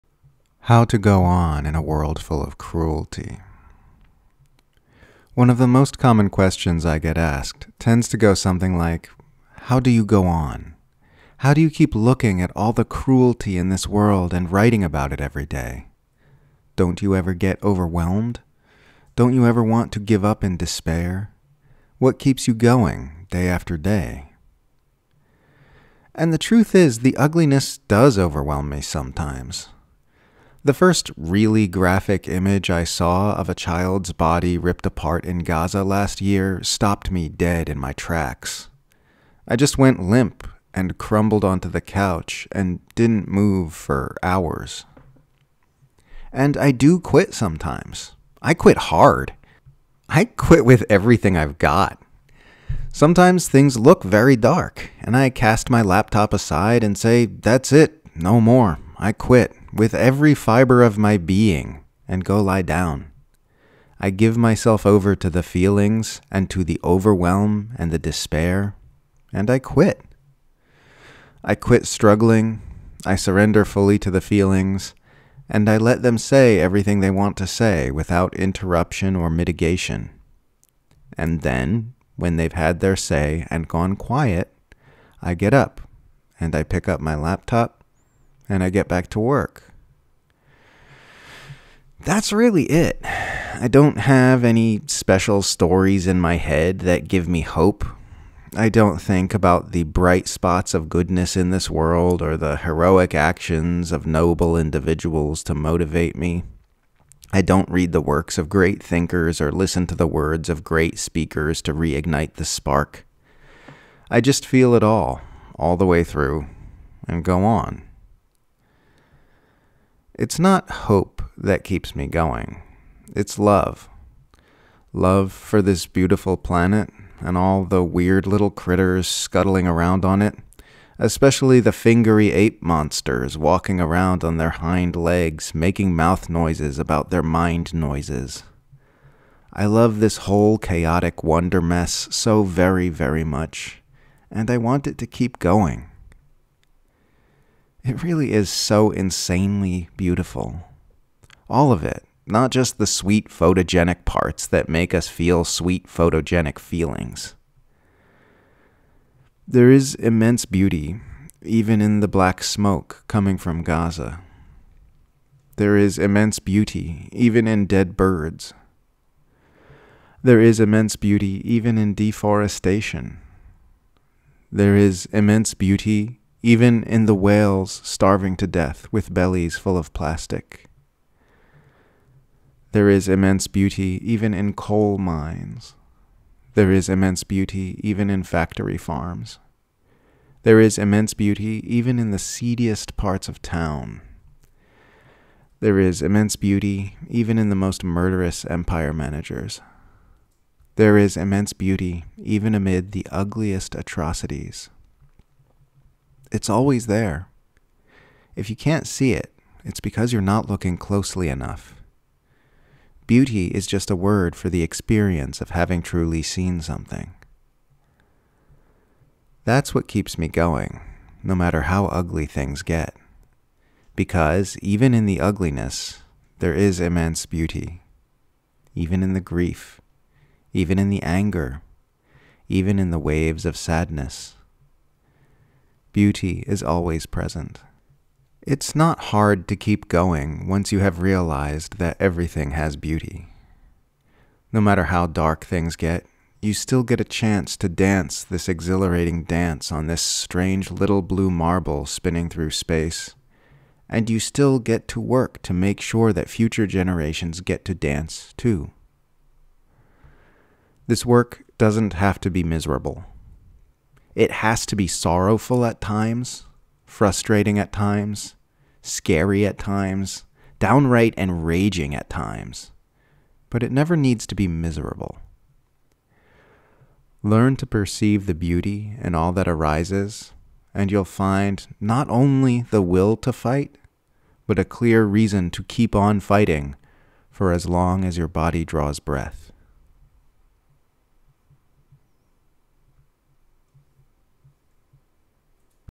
How To Go On In A World Full Of Cruelty
It's not hope that keeps me going, it's love. Love for this beautiful planet and all the weird little critters scuttling around on it, especially the fingery ape monsters walking about on their hind legs making mouth noises about their mind noises. I love this whole chaotic wondermess so very, very much, and I want it to keep going.